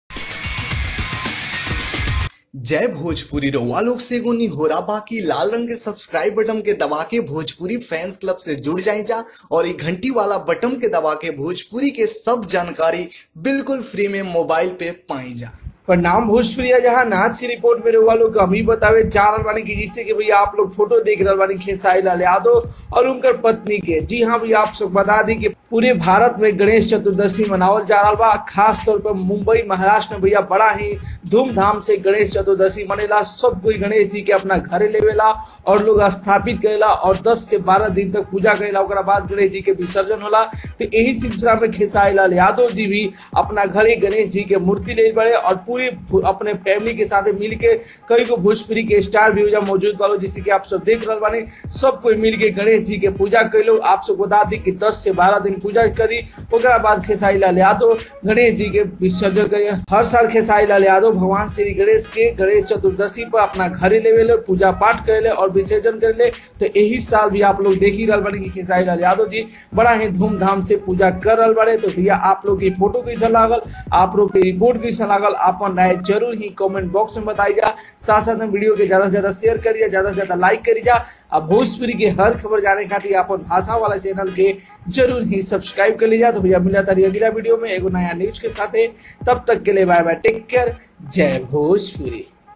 देखिये खेसारी लाल और उनकी पत्नी किस तरह पूजा कर रहे है - Khesari Lal with his wife video
Credit & Thanks : - Khesari Lal Yadav